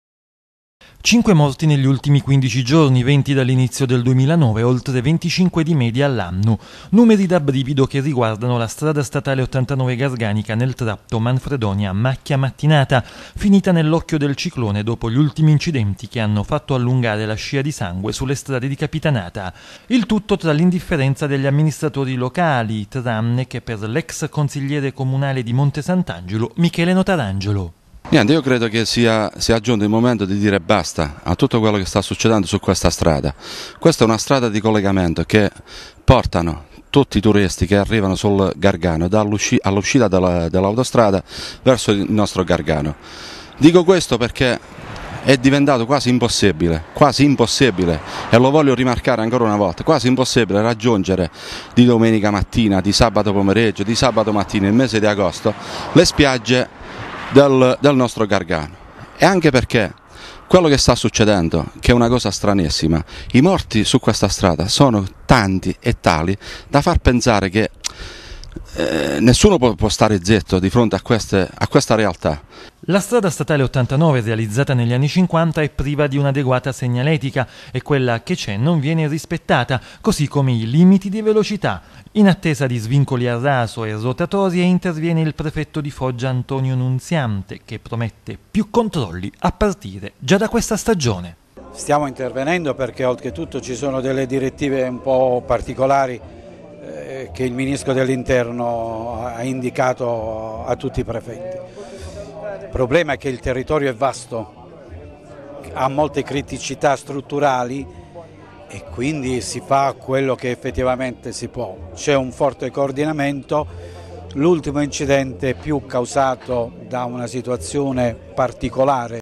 S.S.89 Garganica, la strada della morte
Monte Sant'Angelo. Servizio realizzato da Saverio Serlenga in collaborazione con "il diario Montanaro". Intervista all'ex consigliere comunale Michele Notarangelo che protesta per la scarsa sicurezza su quella che è stata definita "la strada della morte", ovvero quella che porta da Manfredonia a Mattinata, situata a Macchia, nel territorio di Monte Sant'Angelo.